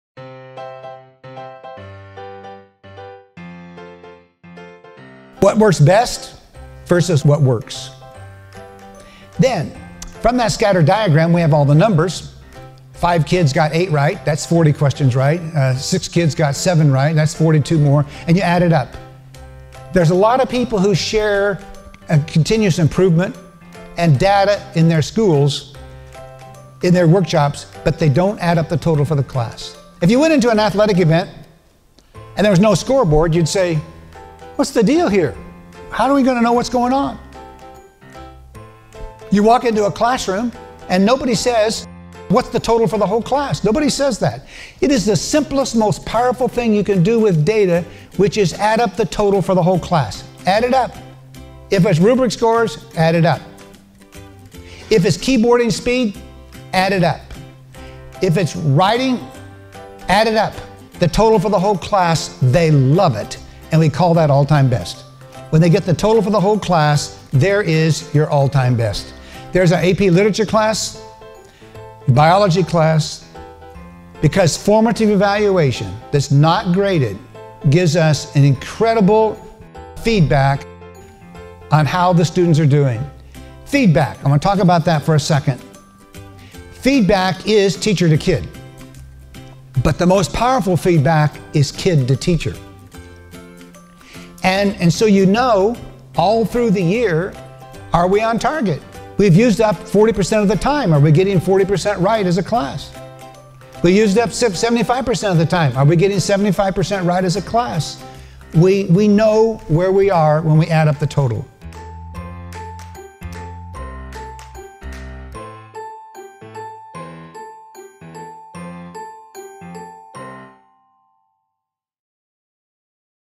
Lee Jenkins' AdvancED Speech - What Works Best
No one is asking what the total is for the whole class, but they should be. Ungraded formative evaluation gives you incredible feedback on how the students are progressing.

Dr. Lee Jenkins is a former teacher and California superintendent who is now a full-time education consultant, speaker, and author. The latest results with his LtoJ® process, from 311 K-12 classrooms, is learning at 5.9 times the average classroom. Think about what happens when kids pressure the teacher to teach more instead of the teacher pressuring the kids to learn more.

Watch the 3.5 minute video entitled “Better Outcomes; Happier Kids.”
Read the preface, introduction and first two chapters of his upcoming book